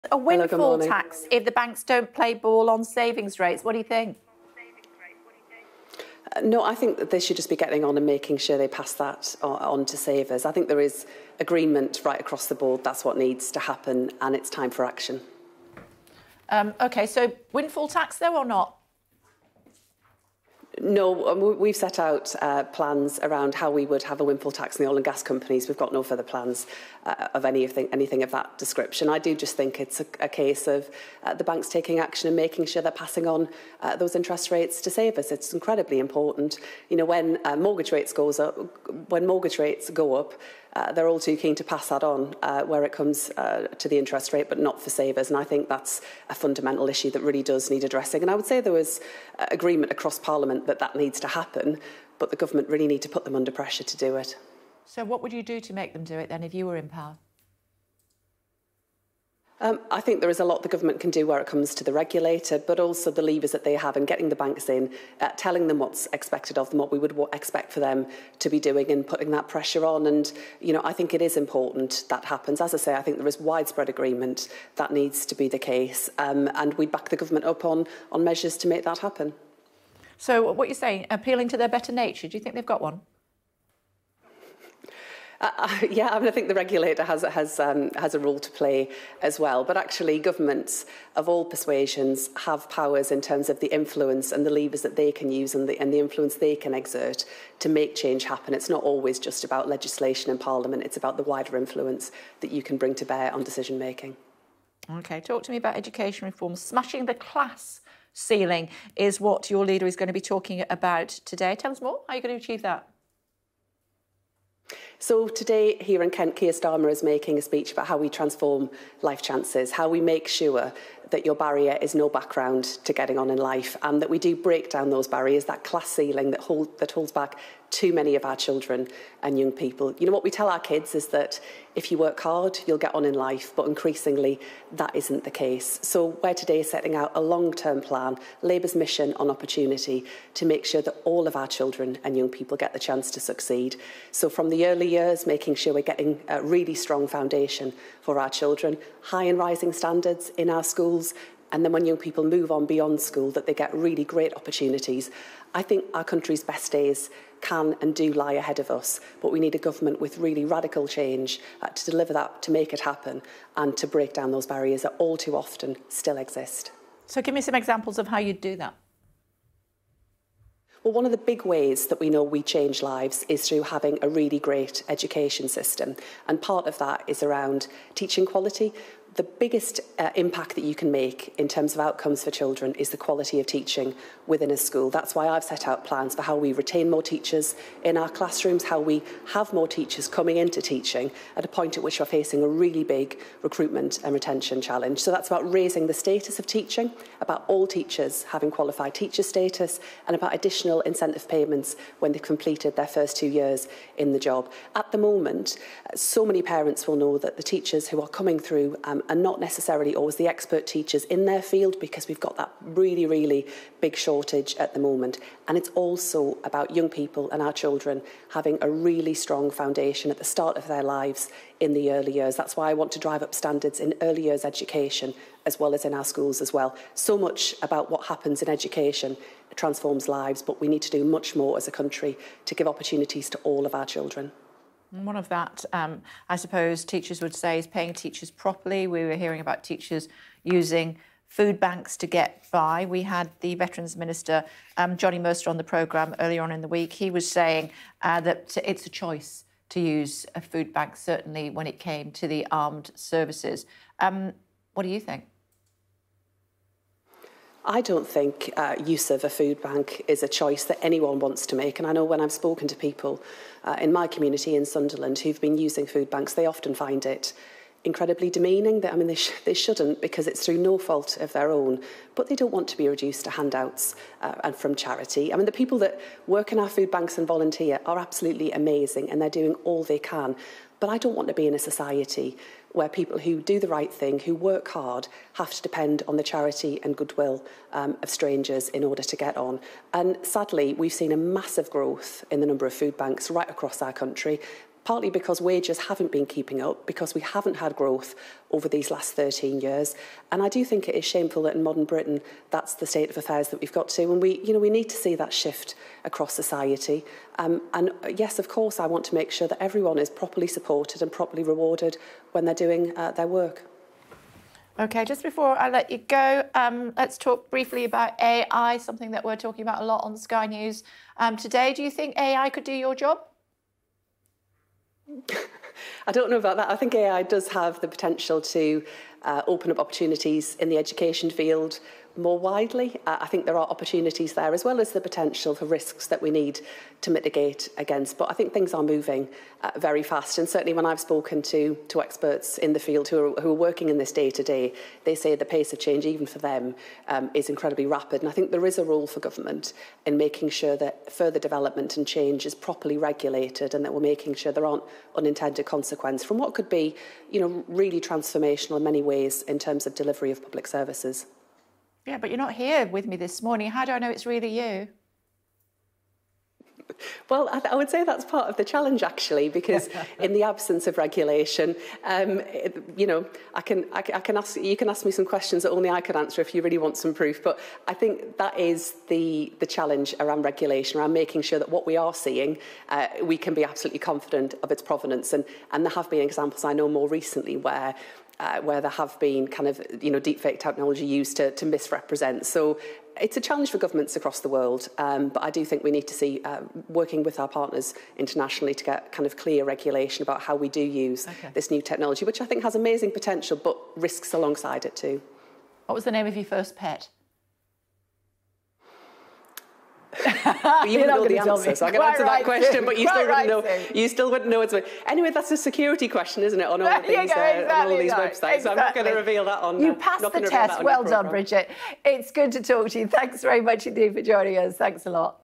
Bank Rates: Windfall tax plan is not for banks - Labour
Shadow education minister Bridget Phillipson today told Sky News that Labour's plan for a windfall tax would be for oil and gas companies rather than banks.

It comes as major banks have been accused of profiteering by failing to increase the saving rate in line with interest, despite hiking mortgage costs.

A number of banks will appear before the Financial Conduct Authority today.